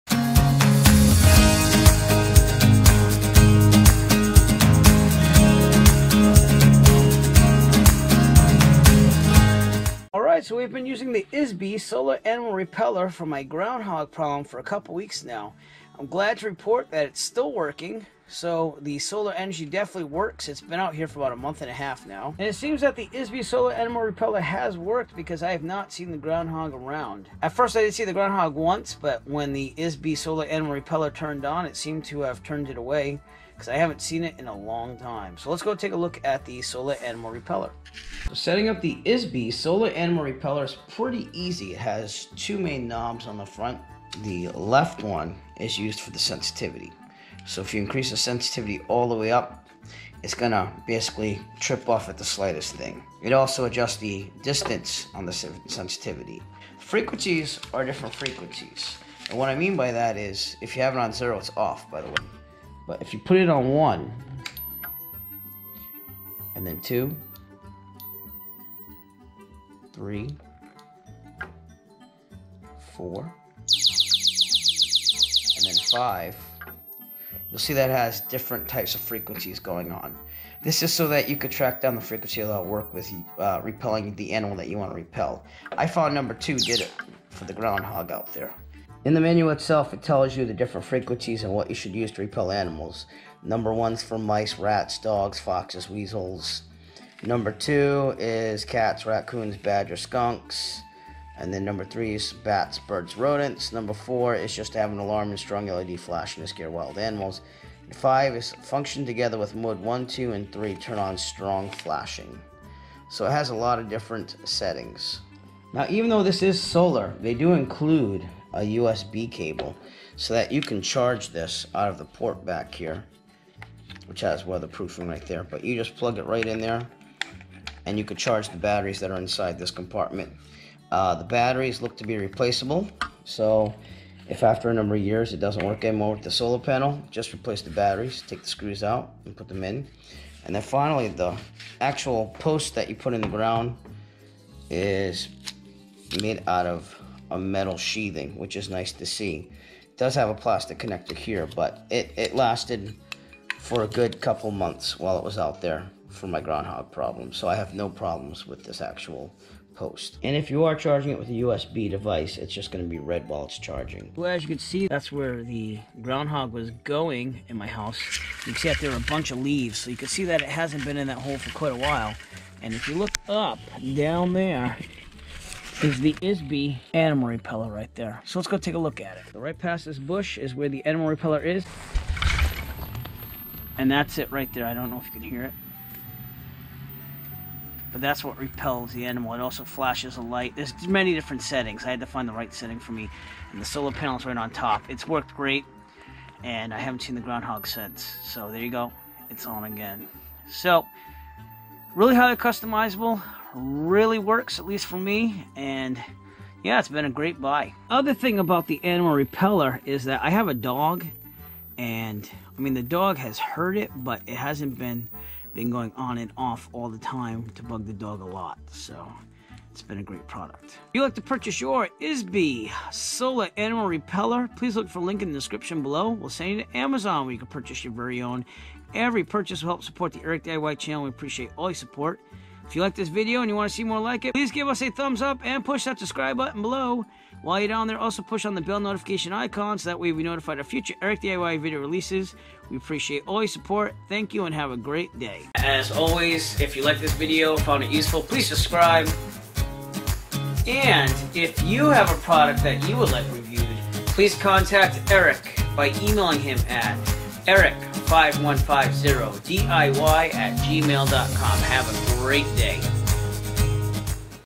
Izbie Z1 Solar Ultrasonic Animal Repeller Review
Buy the Izbie Solar Ultrasonic Animal Repeller reviewed in this video on Amazon
(paid ad)

Buy the GoPro Hero 7 Black camera that we use to film this video in 4K 60fps
(paid ad)

We had a groundhog problem here at our house and I was out of options. I tried garlic and other things while also plugging up holes. I finally found the Izbie Solar Animal Repeller and it seemed to help a lot. I only saw the groundhog once after installing and then I never saw him/her again. It's really easy to use, quick setup, and charges itself during the day (I never had it run out of power while using it). If you have an animal problem, I do recommend trying the Izbie Solar Ultrasonic Animal Repeller.